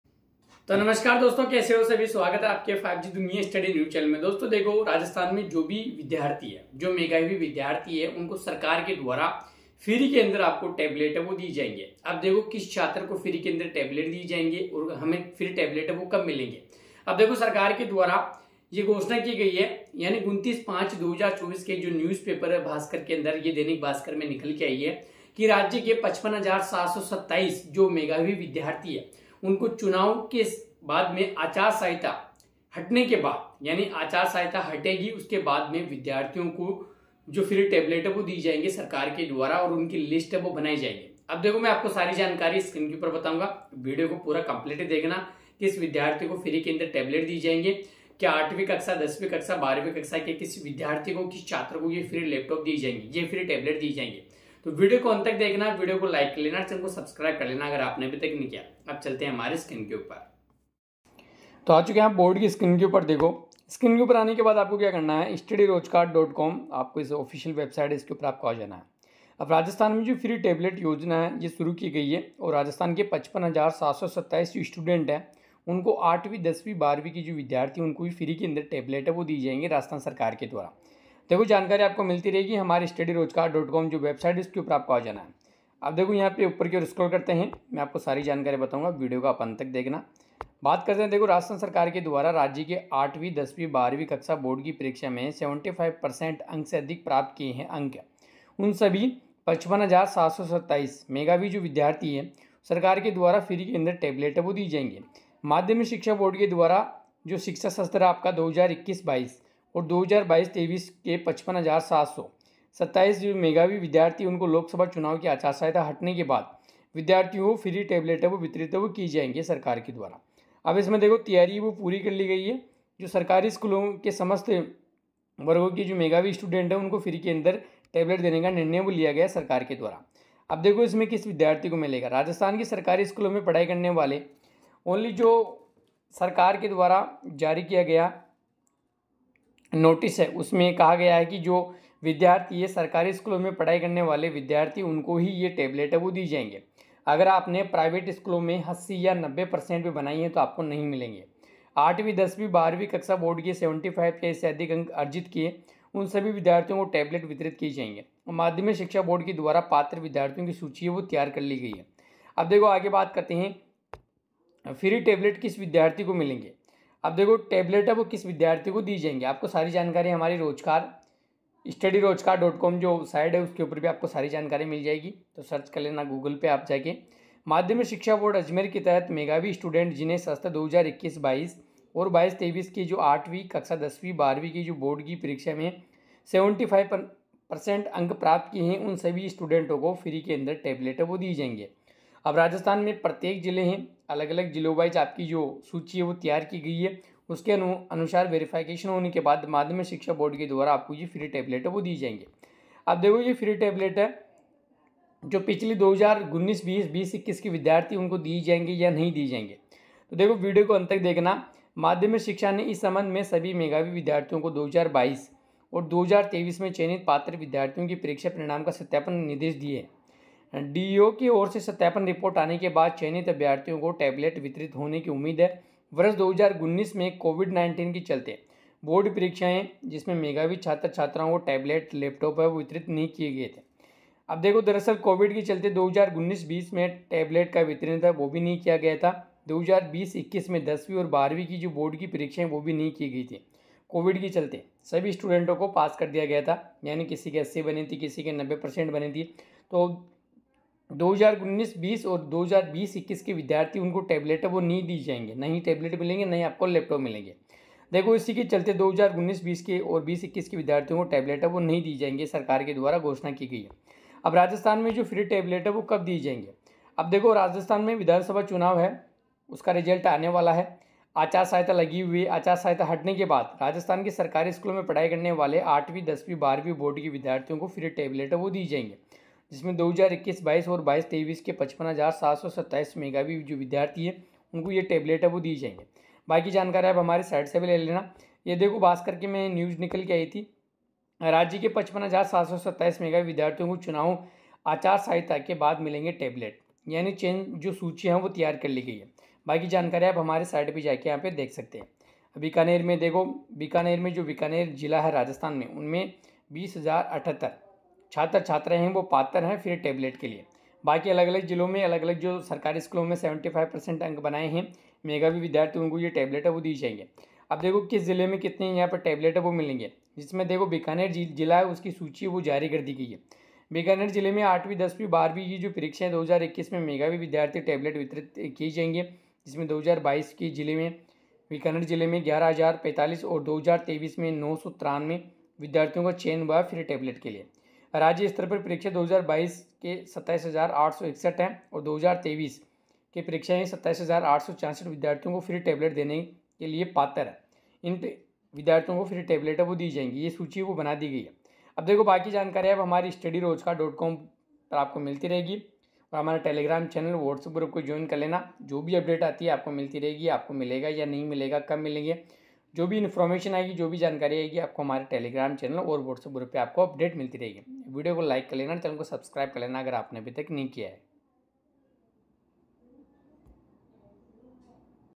फ्री टैबलेट योजना 2024 | rajasthan free tablet kab milega | free tablet kis student ko milega
टैबलेट योजना 2024 | rajasthan free tablet kab milega | free tablet kis student ko milega

राजस्थान फ्री टैबलेट कब मिलेगा
राजस्थान में मिलेंगे 55,727 विद्यार्थियों को फ्री टैबलेट
सरकार देगी 8वी 10वी 12वी के विद्यार्थियों को फ्री टेबलेट फ्री टैबलेट योजना की लिस्ट जारी